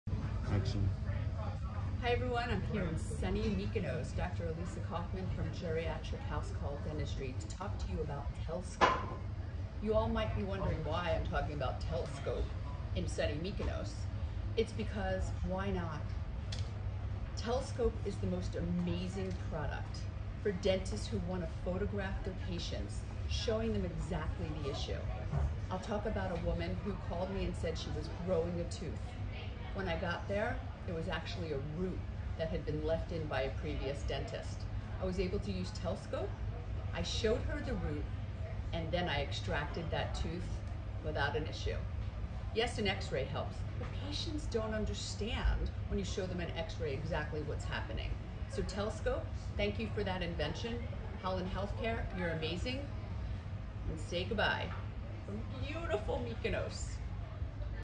Finding a Tooth Root Tip Left After Extraction in a Geriatric Patient
Dr. Alisa Kauffman from Geriatric House Call Dentistry had a geriatric patient who complained of a new tooth growing through and causing pain.